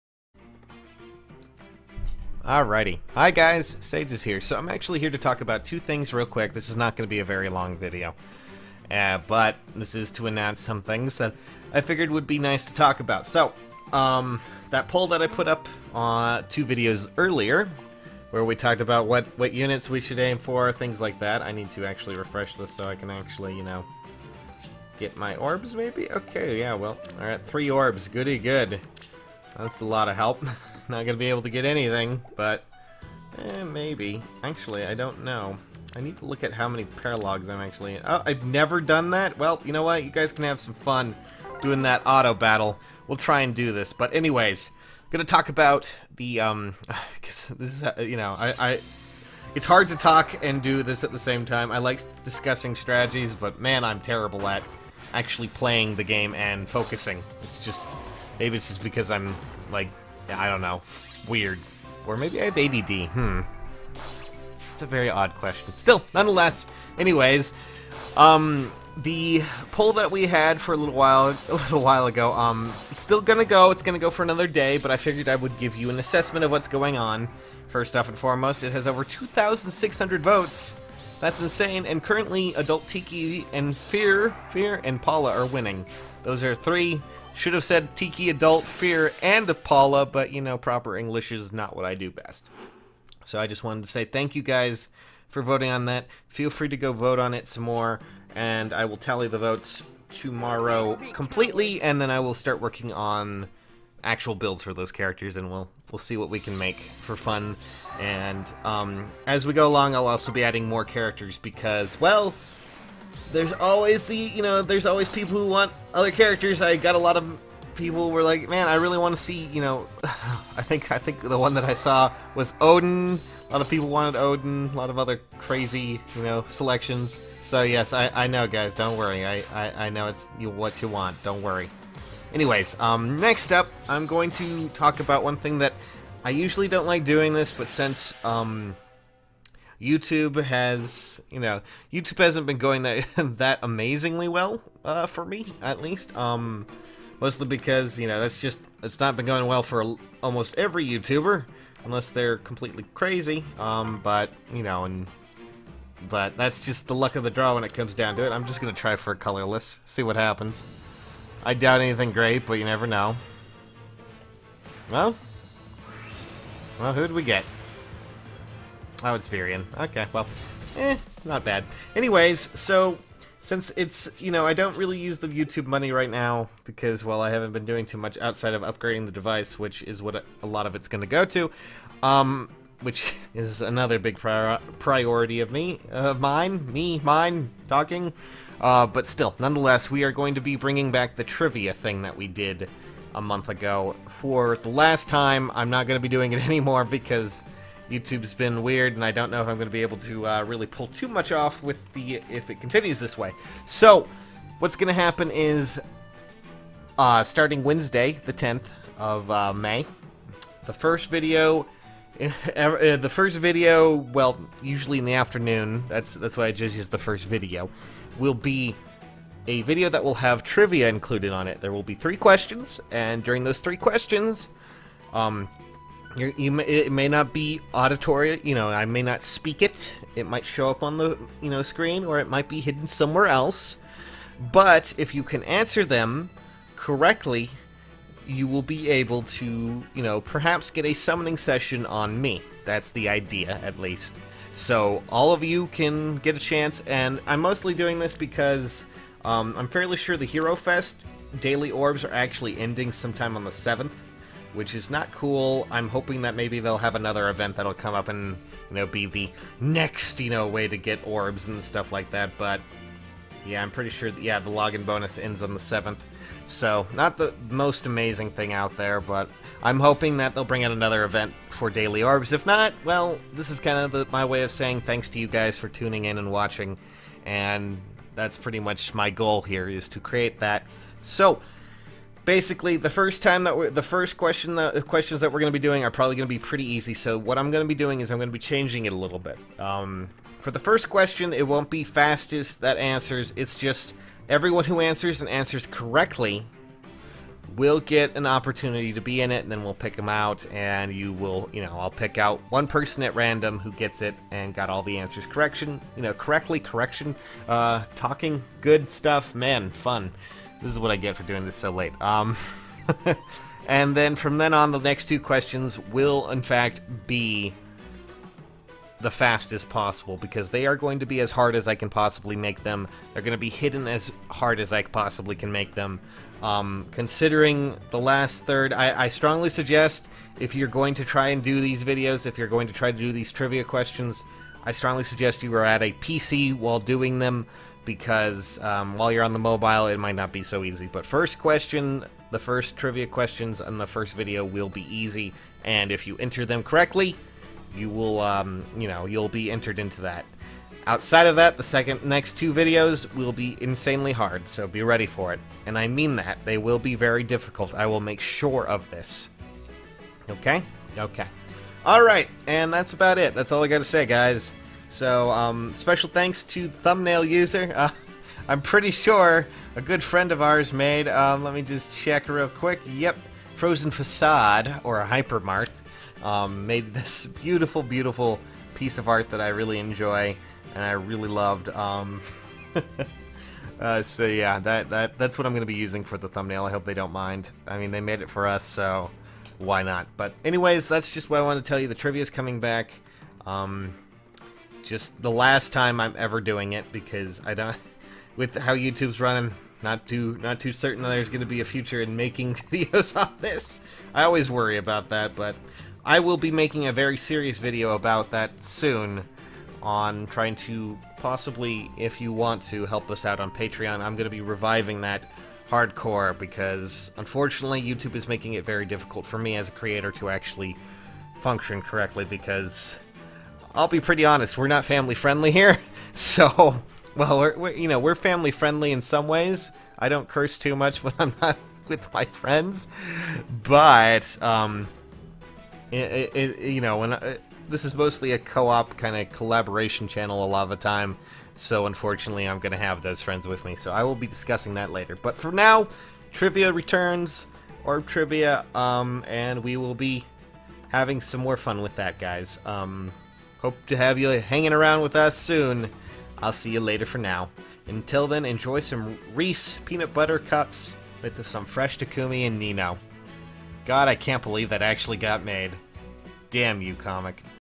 TRIVIA ORBS RETURN FOR THE LAST TIME
I figured with the last good paycheck I'm getting out of youtube, we'd revive the fun of the trivia one last time. So, here we go again.